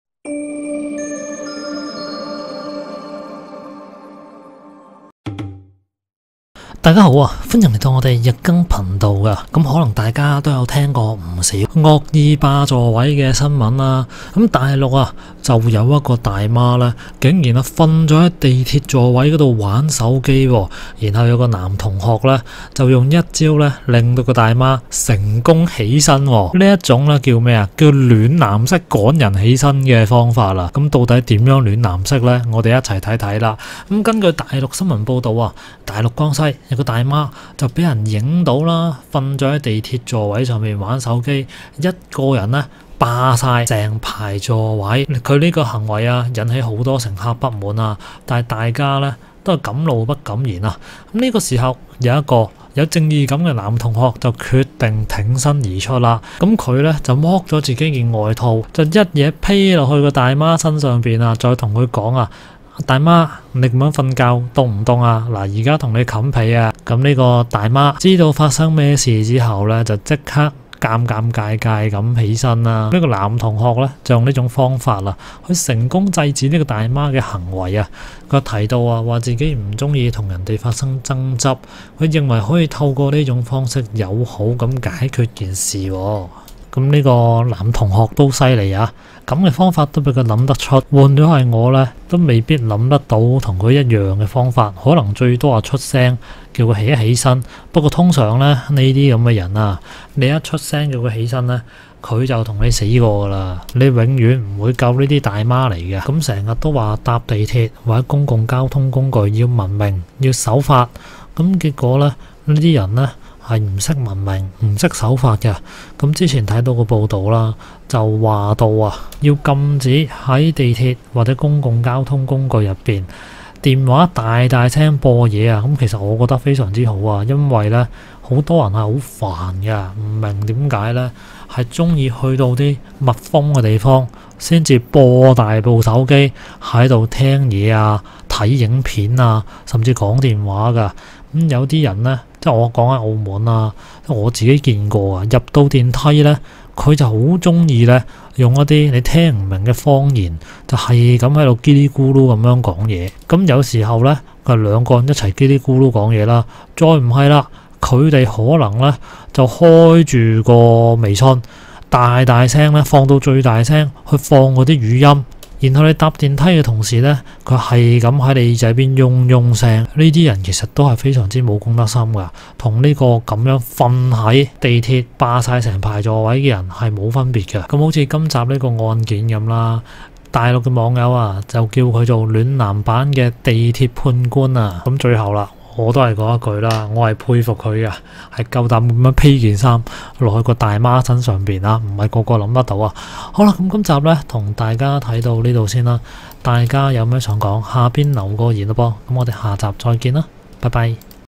🔴用魔法打敗魔法！大陸惡劣大媽躺平地鐵座位滑手機！男同學用1招成功反制！暖男版地鐵判官出現了！｜CC字幕｜Podcast｜日更頻道
在搭乘大眾運輸時，難免會遇到一些自私乘客。大陸江西一名大媽被拍下橫躺在地鐵座位上滑手機，引來其他乘客側目，敢怒不敢言，有位充滿正義感的男同學見狀後，隨即把外套脫下，轉而蓋在大媽身上，禮貌問「您這樣睡覺冷嗎？」，大媽立刻會意並連忙起身。影片曝光後，男同學的高情商獲網路一片讚聲。